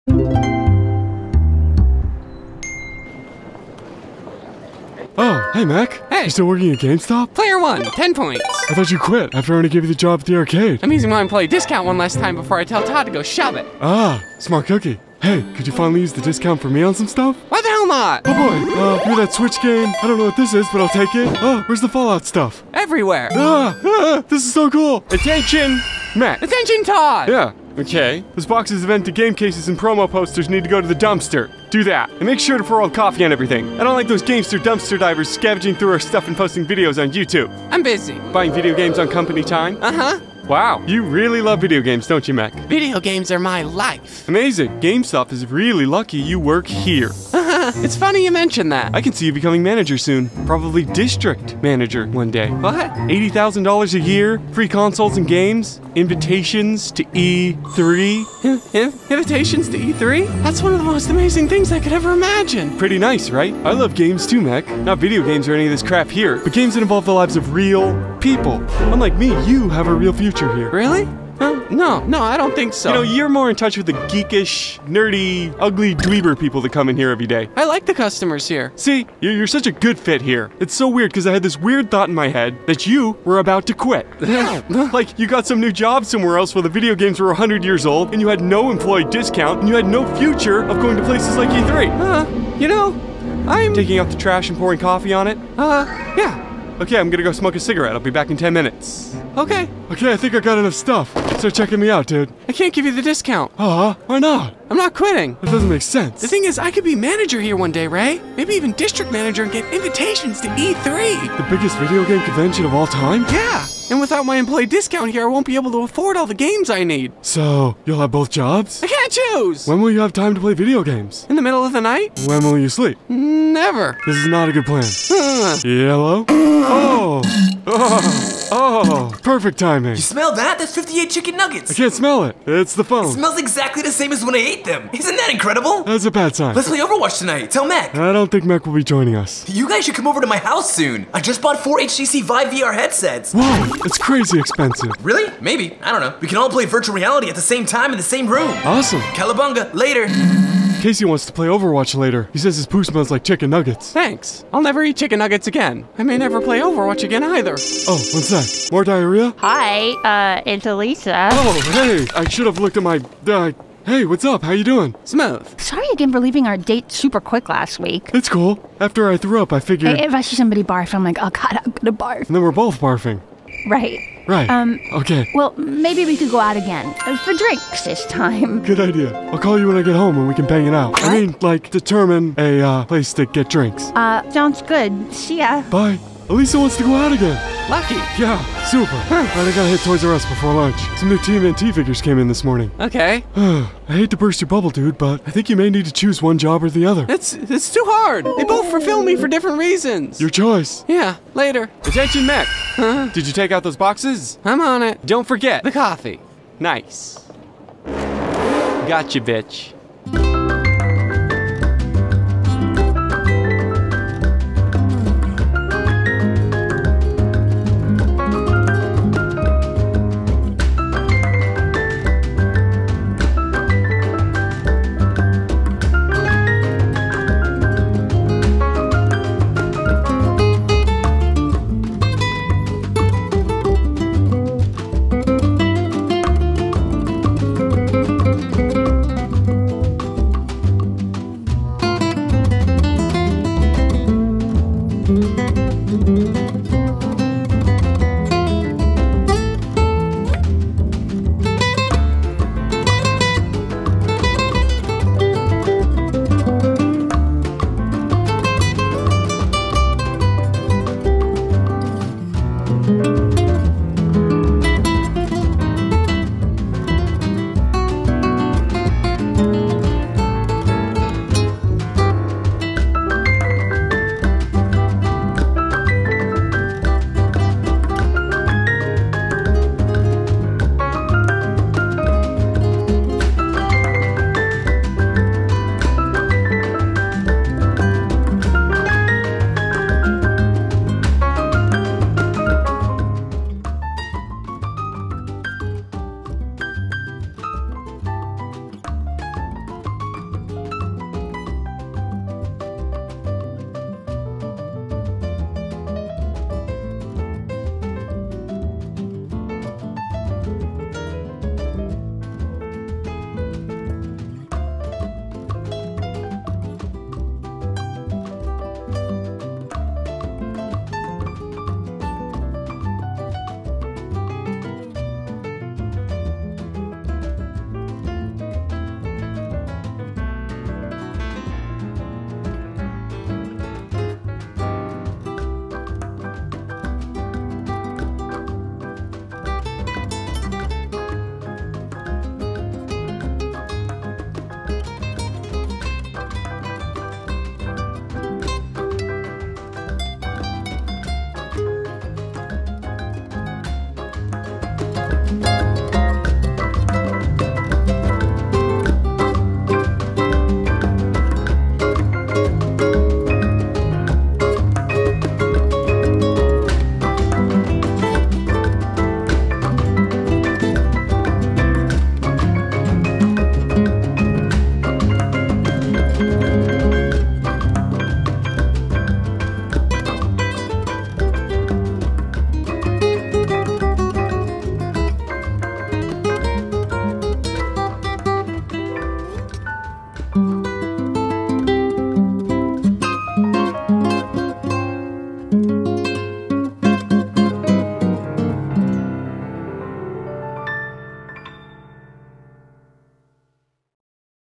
"E3?" - Ray and Meck - EPISODE 20
MORE CAST LINKS!

EPISODE 20:
Meck is going to quit GamesTop?